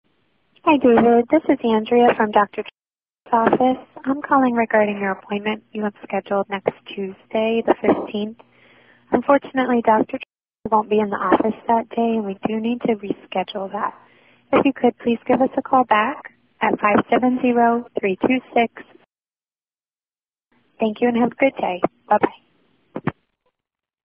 Doctor's Office Voicemail - American English Listening Practice
An example of a real voicemail from an American doctor's office.